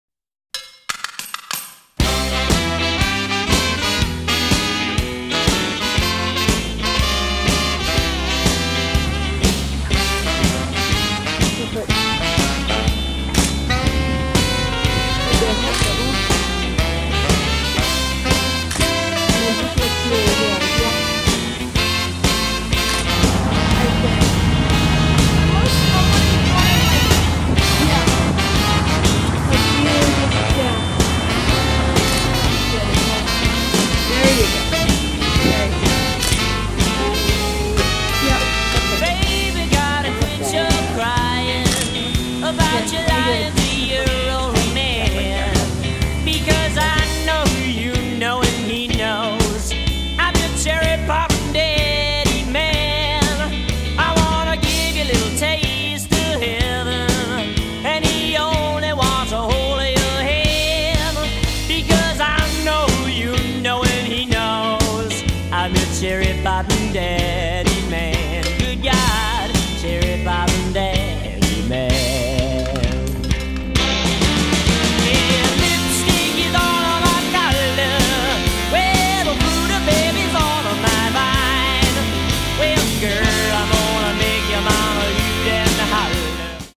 TURA SATANA HOMAGE
Dejah Garcia and Justice do homage to Tura Satana of Faster Pussycat Kill Kill cult movie fame